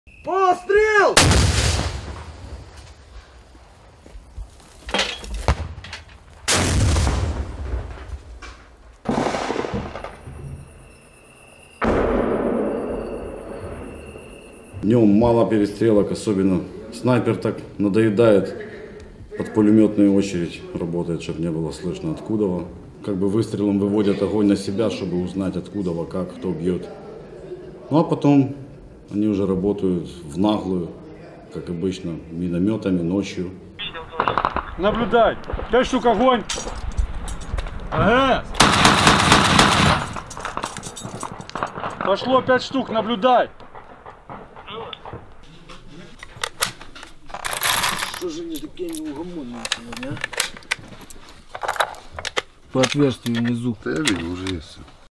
За последние сутки на Донбассе погиб один украинский военнослужащий, 11 получили ранения
О своих потерях боевики непризнанных "ДНР/ЛНР" не сообщают. Эти кадры мы сняли минувшей ночью в районе Марьинки Донецкой области.